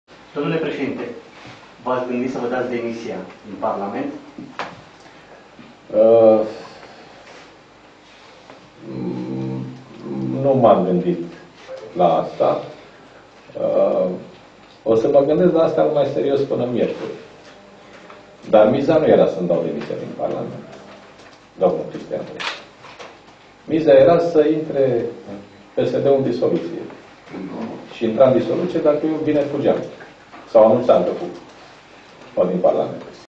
Conferință de presă după ședința Biroului Permanent Național al PSD din 25 aprilie 2016
Liviu Dragnea: Miza nu era să-mi dau eu demisia, ci să intre PSD-ul în disoluție și intra în disoluție dacă eu vineri fugeam.
Titlul evenimentului: Conferință de presă după ședința Biroului Permanent Național al PSD din 25 aprilie 2016